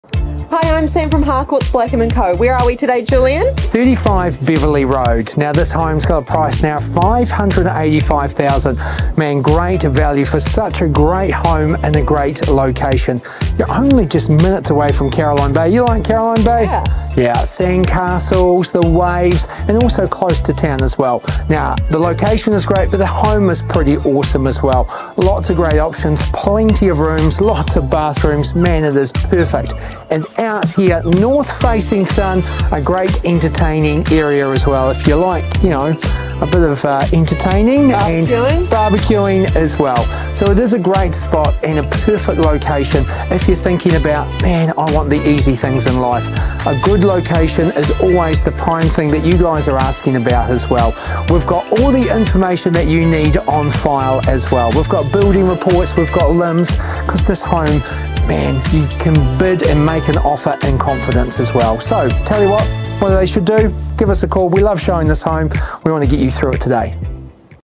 35 Beverley Street, Maori Hill | Team Blanchard
This appealing four-bedroom home, purchased just 18 months ago, is back on the market due to the current owners' job relocation and desire to be closer to family. In their short tenure, the owners have lovingly made enhancements, further elevating the property's charm and functionality.
The residence strikes a perfect balance between comfortable living spaces and thoughtful updates, ensuring a turnkey solution for the next lucky homeowner.
Situated within walking distance to the picturesque Caroline Bay, the location offers both convenience and leisure opportunities right at your doorstep.
The owners, keen to sell, present a fantastic opportunity for those looking to settle in a sought-after area.
With reports on file and settlement, deposit and finance terms to suit, contact Julian or Sam today to view.

(copy and paste the link into your browser)